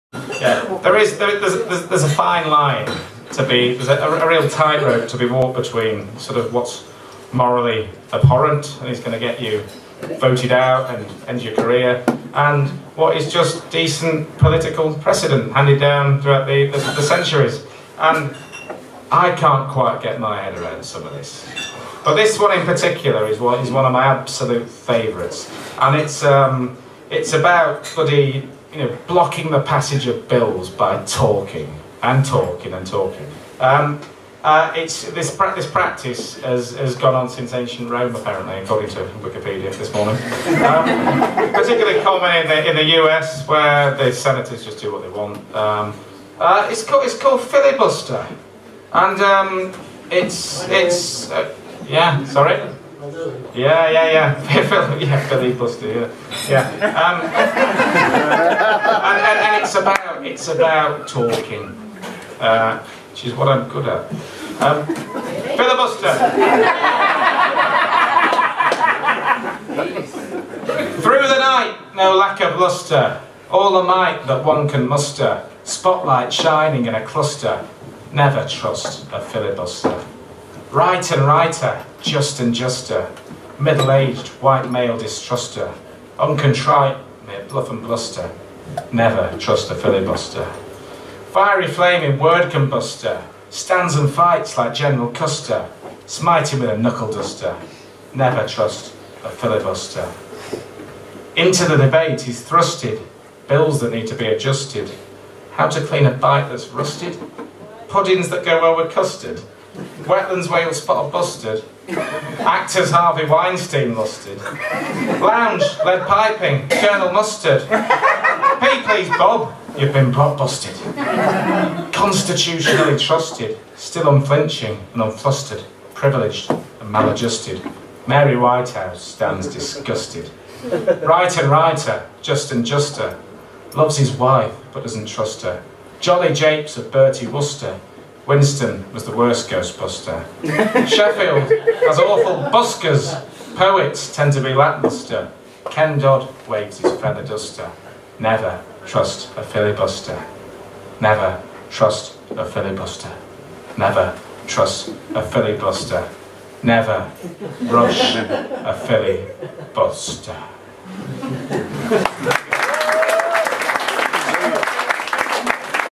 Filibuster - Kevin Owers
When it's time to stop talking.

‘Office Politics verses. Domestic Affairs’.
Kev’s charity gig for The Marmalade Trust, performing his particular brand of throwaway poetry live at the Dorothy Pax, Sheffield, on Monday 2nd December 2019.

If you like what you see, buy a book!